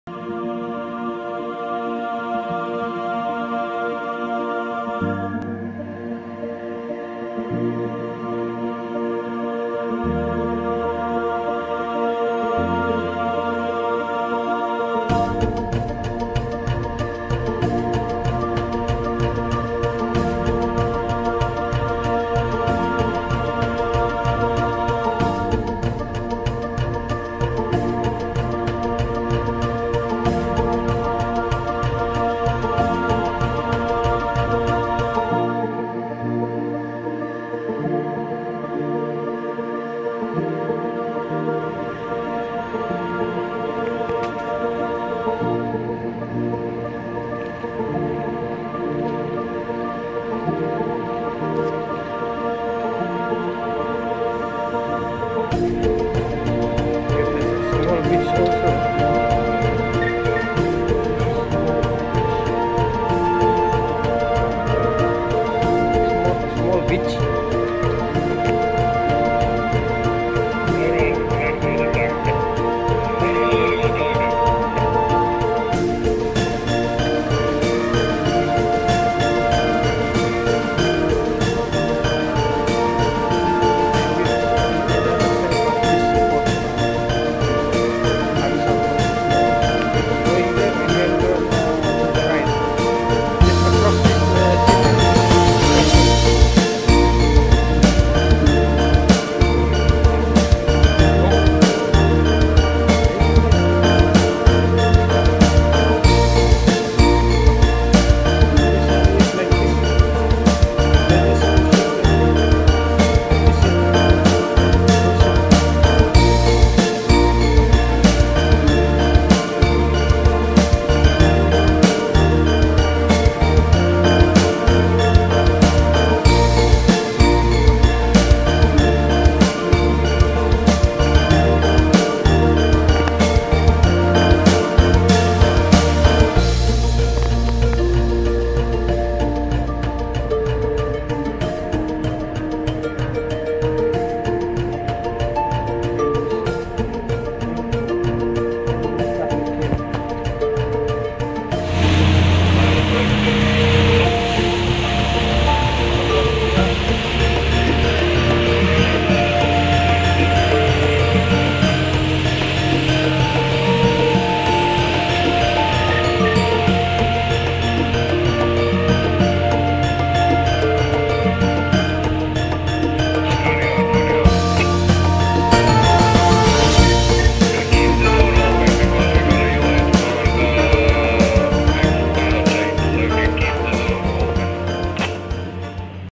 Sailing from Hamburg
Ship sailing from Hamburg to Lithuania
North Europe voyage not Finish yet